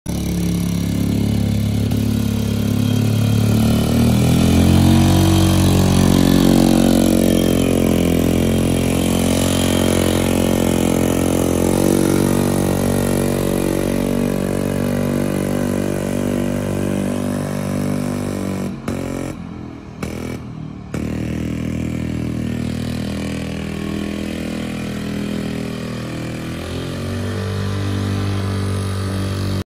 1969 Harley Davidson Golf Cart
short ride on HD Golf Cart.   Runs great, forward and reverse.  Former Shriner's parade Cart